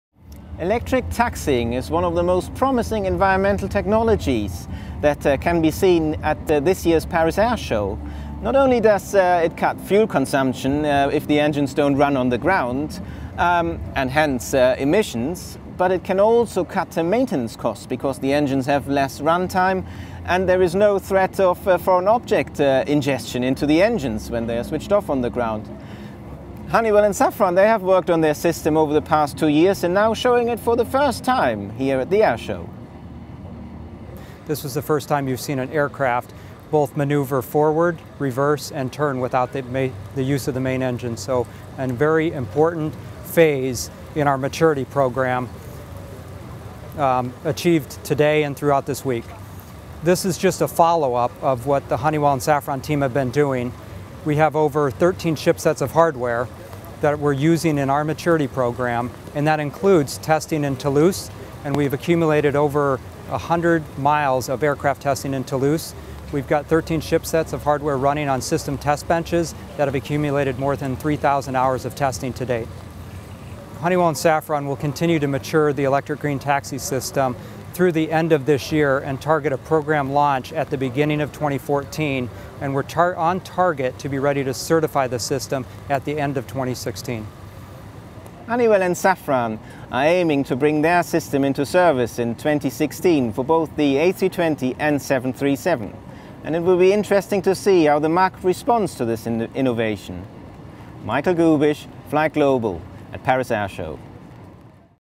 Flightglobal looks at the Honeywell/Safran Electric Green Taxiing System joint venture
We talk to the EGTS programme vice president Brian Wenig about the Electric Green Taxiing Systems cost savings and benefits.